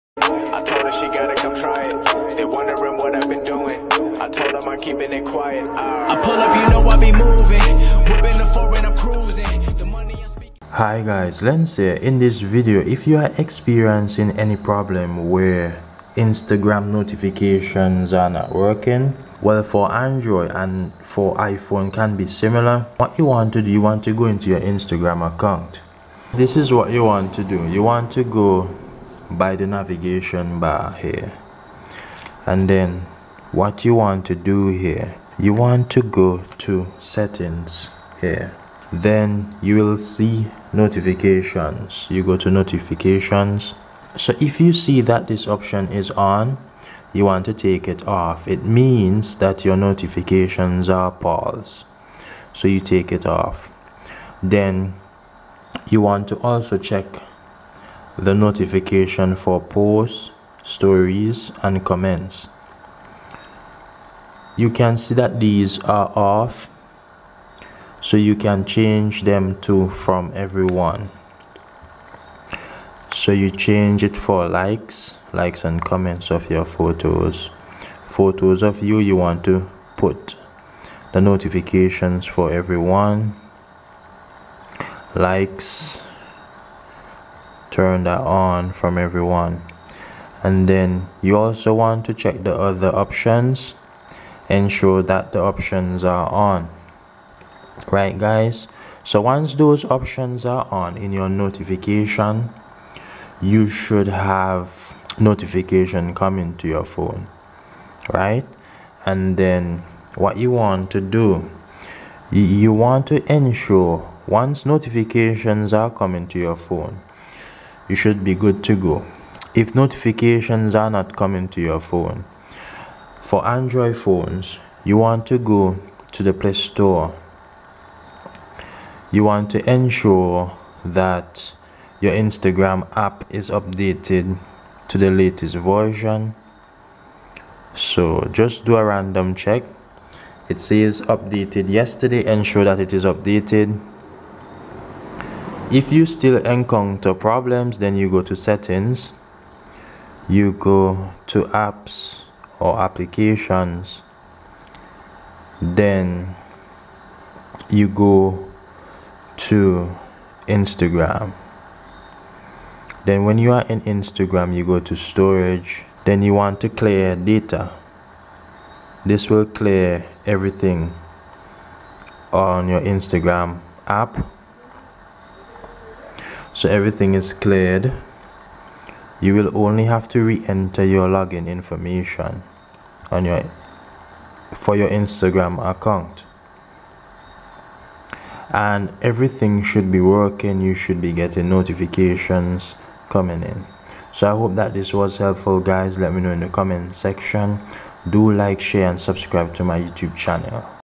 How to fix Instagram notification not working for android and iPhone
Fix Instagram Notifications Not Working on Android and iPhone

    Restart Your Phone. ...
    Logout from Your Account. ...
    Update Instagram App. ...
    Turn off Do Not Disturb Mode. ...
    Check Notification Settings in Instagram App. ...
    Check Notification Settings from a Different Phone. ...
    Check Instagram Notification Settings on Your Phone.